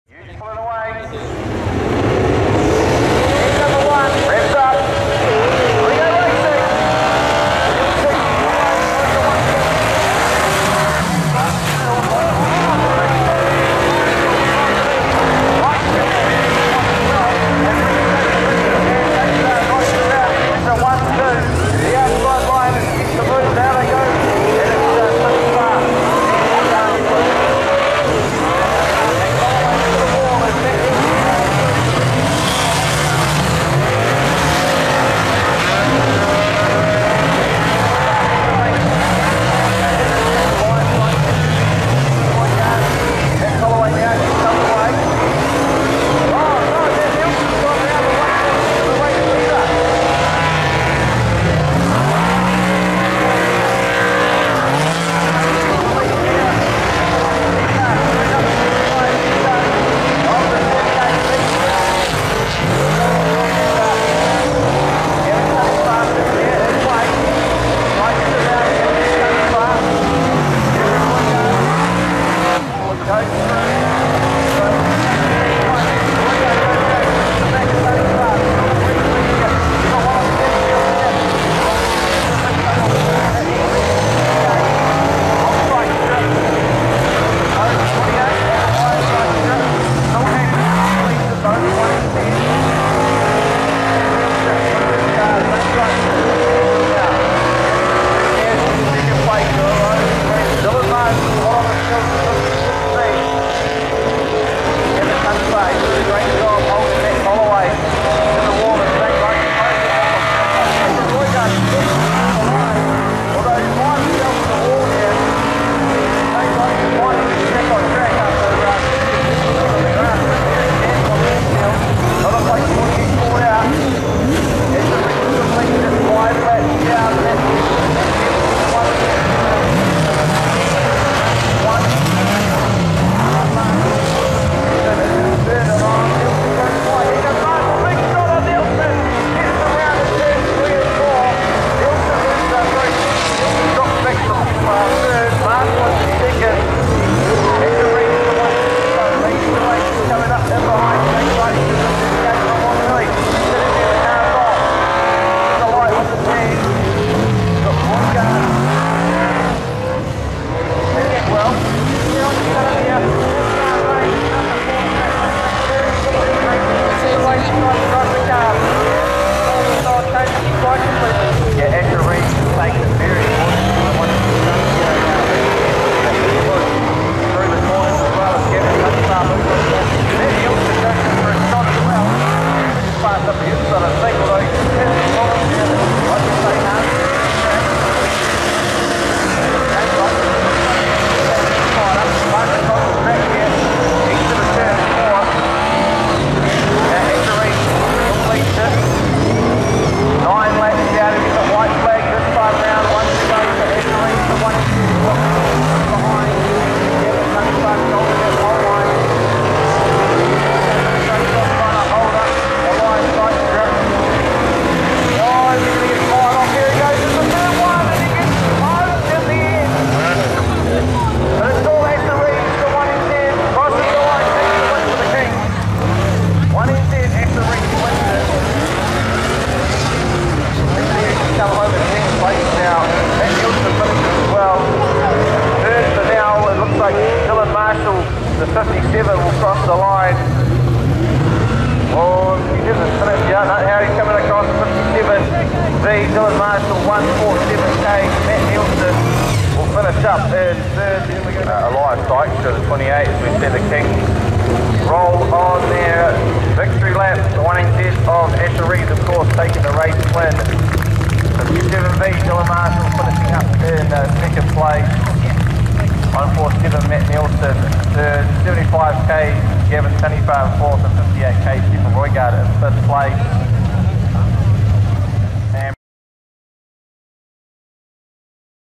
Waikaraka Park Teams Qualifying - Race 1 - Kihikihi v Whanganui - Sat 24.04.21 (4K)
Teams Race 1 (Kihikihi Kings v Whanganui Warriors) of the Qualifying night action for the Auckland Teams Nationals meeting held at Waikaraka Park Speedway on Saturday 24 April 2021